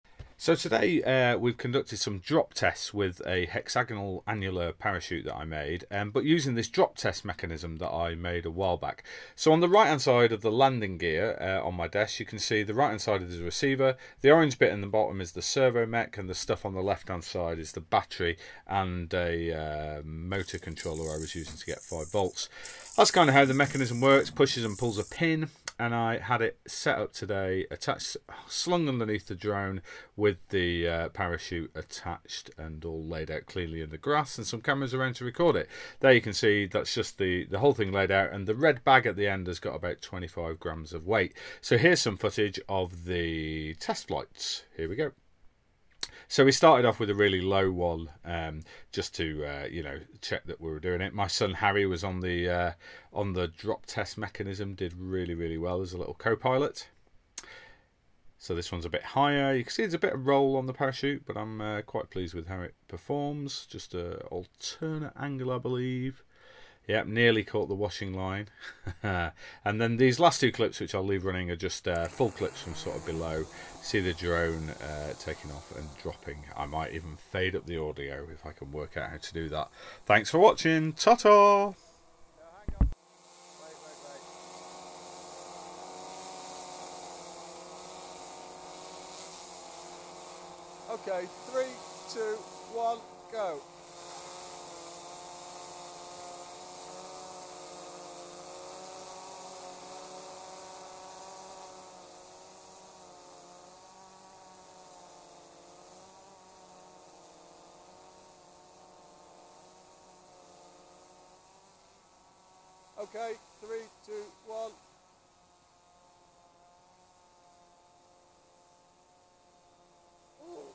Drop testing a DIY hexagonal annular parachute from drone
A quick and dirty edit together of the drop test mechanism I put together to allow me to drop test parachutes off a drone. Here me and my son are testing out a Hexagonal Annular experimental parachute design that I made a while ago. For more details and links to parts of the drop mechanism and the parachute design check out my blogpost here